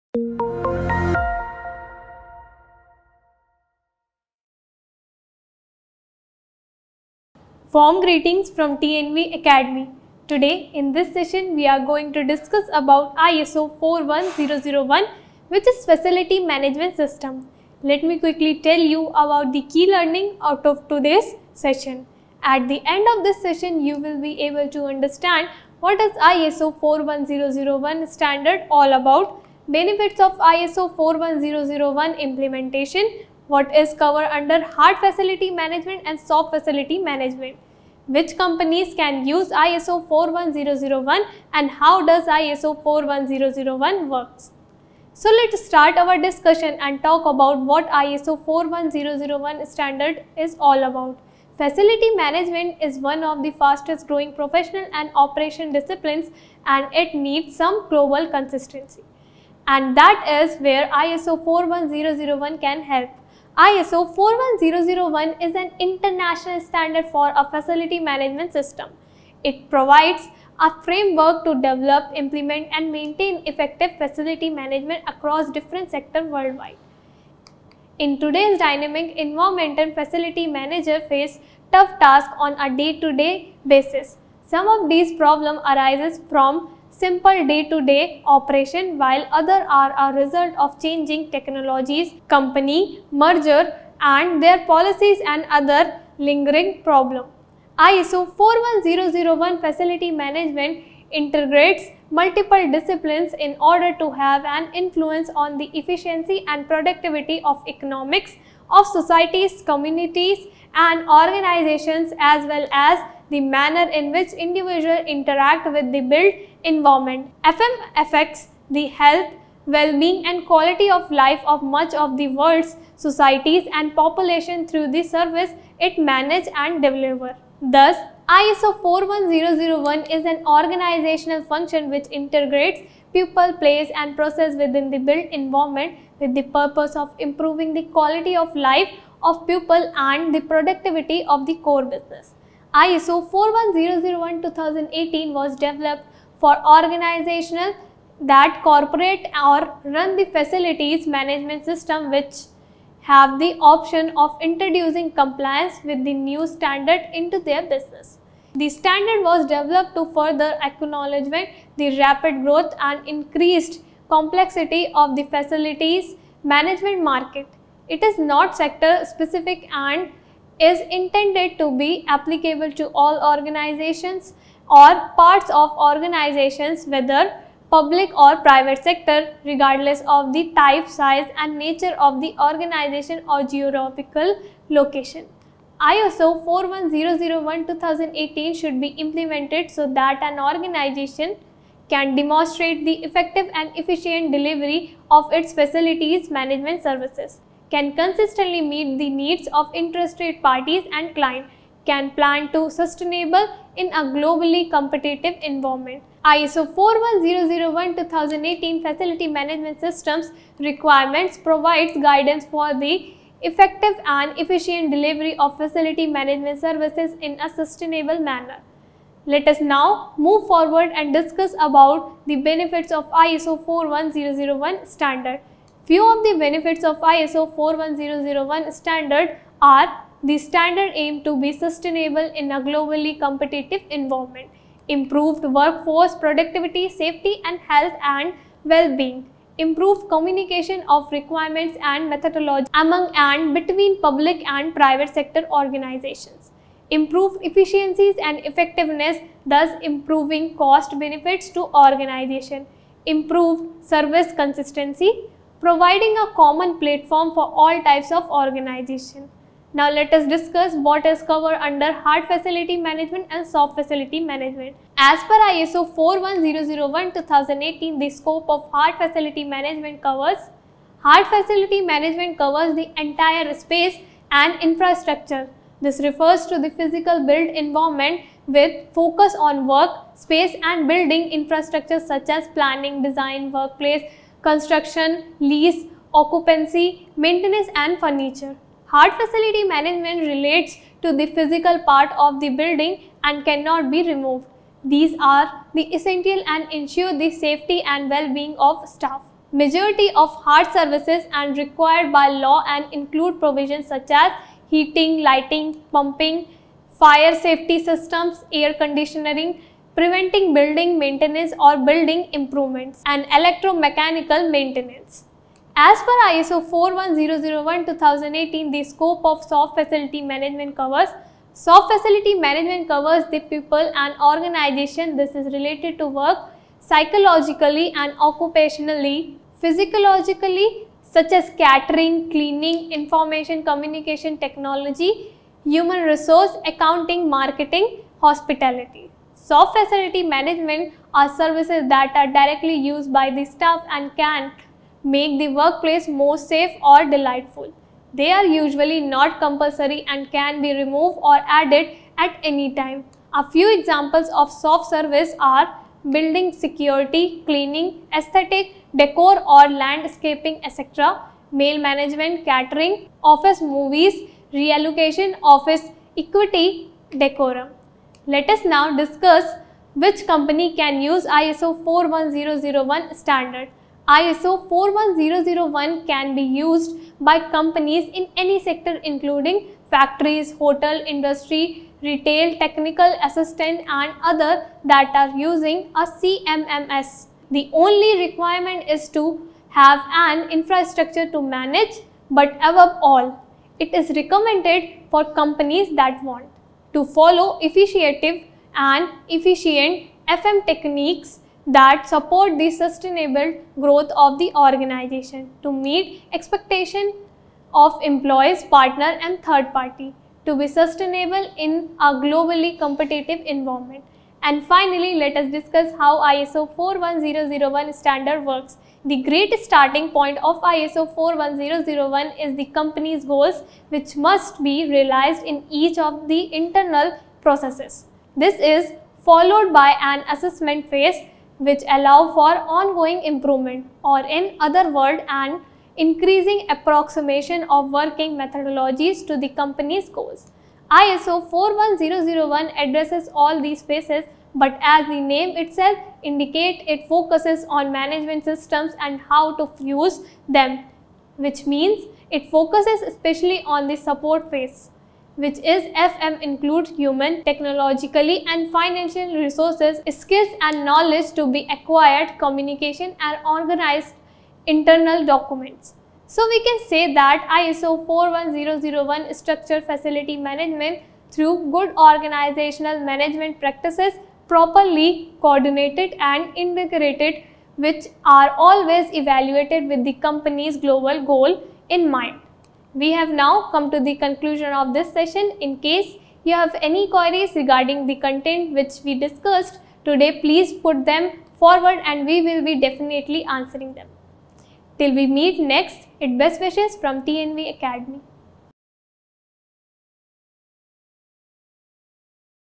What is ISO 41001 Facility Management System?
In This session we have discussed about ISO 41001 which is Facility Management System. We have discussed about What is ISO 41001 Standard all about, Benefits of ISO 41001 Implementation, What is Covered under Hard Facility Management and Soft facility management, Which Companies can Use ISO 41001, How does ISO 41001 Work.

 ISO 41001:2018 is “Facility Management System”

ISO 41001 is the international standard for a Facility Management (FM) system. It provides a framework to develop, implement and maintain effective facilities management across different sectors worldwide. ISO 41001 Facility Management (FM) integrates multiple disciplines in order to have an influence on the efficiency and productivity of economies of societies, communities and organizations, as well as the manner in which individuals interact with the built environment. FM affects the health, well- being and quality of life of much of the world’s societies and population through the services it manages and delivers.

ISO 41001 is an organizational function which integrates People, Place and Process within the built environment with the purpose of improving the quality of life of people and the productivity of the core business. ISO 41001:2018 was developed for organizations that incorporate or run facilities management systems which have the option of introducing compliance with the new standard into their business.